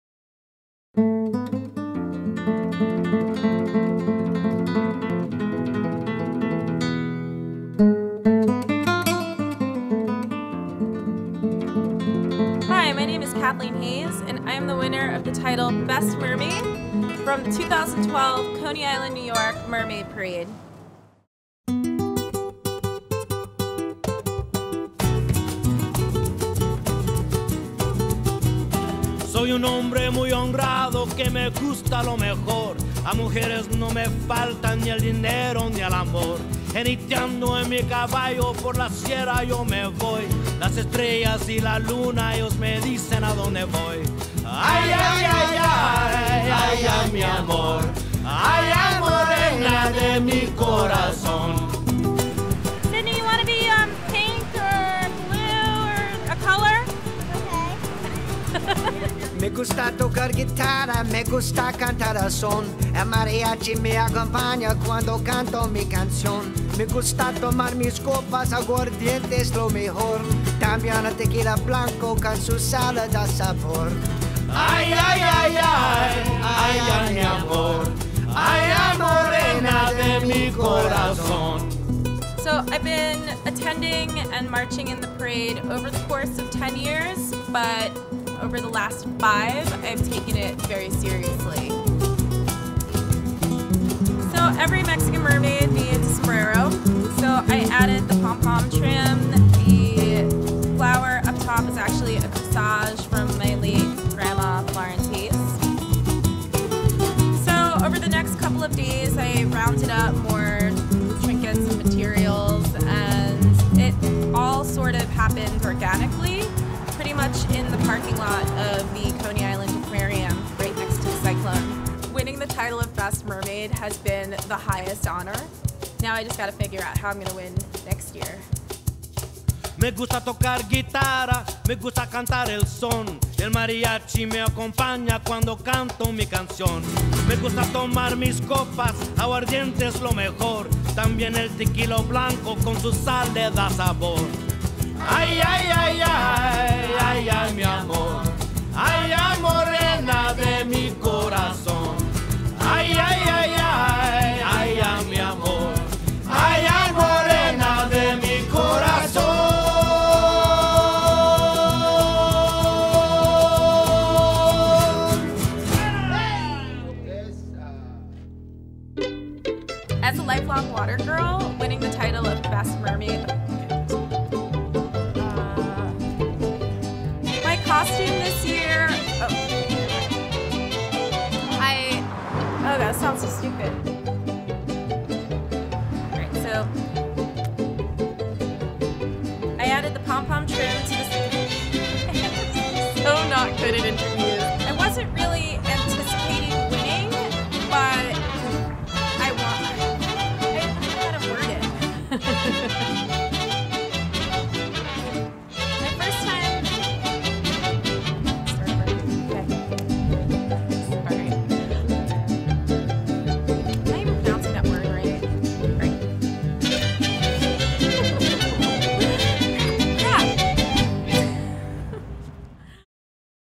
A Portrait of a Mermaid
Here's a short portrait of Kathleen Hayes this year's winner of the Coney Island Mermaid Parade. sweetproductionmedia.com